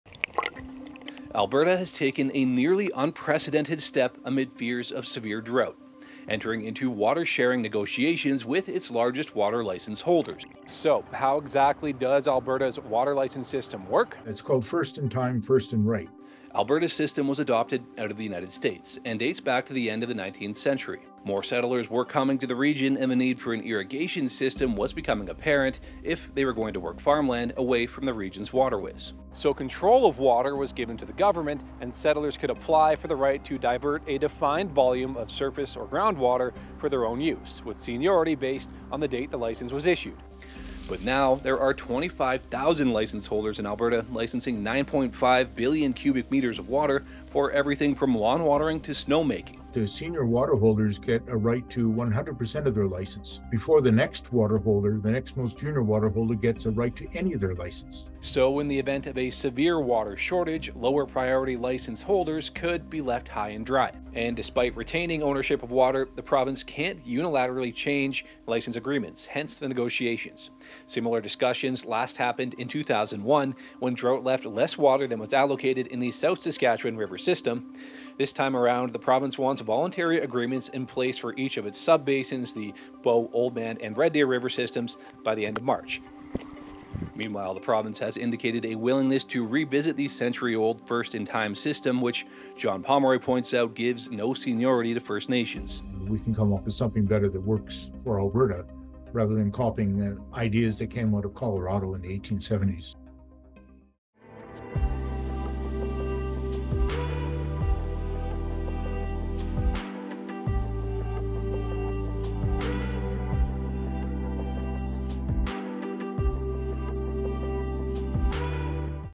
Alberta Enters Water Licensing Negotiations Amid Drought Fears
A centuries-old system of licensing water is due for an update amid higher demand and worsening droughts as some license holders face being left literally high and dry. The Weather Network's Connor O'Donovan explains.

Never miss a weather alert!